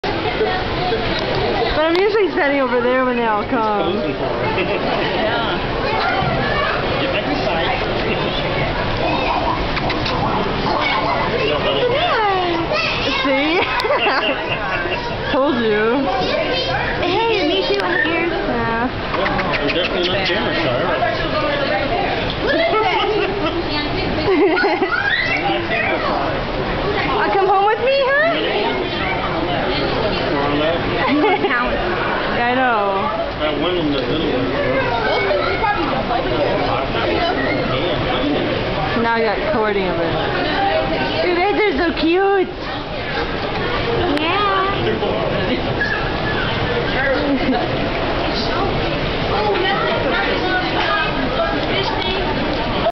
Meerkat Checkout
These Meerkats at the zoo were just SO curious about my 'cat ears' I was wearing :3! These little dudes are one of my favorite animals to check out at the Zoo!